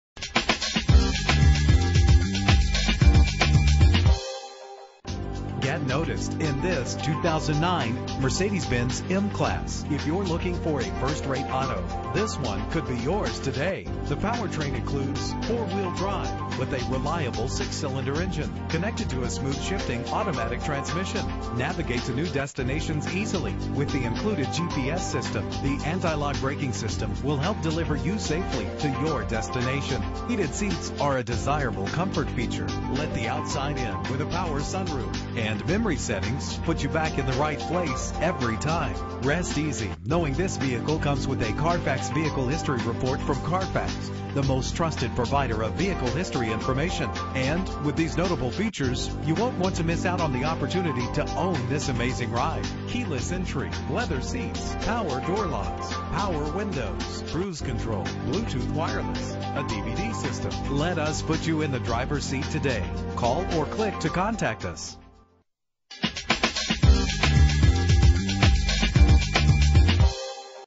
2009 Mercedes-Benz M-Class - Daphne AL
Year: 2009
Make: Mercedes-Benz
Model: M-Class
Trim: ML350 4MATIC
Engine: 3.5 liter 6 cylinder 24 valve
Transmission: 7-Speed Automatic
Color: Iridium Silver Metallic
Mileage: 76475
Address:
27161 Hwy. 98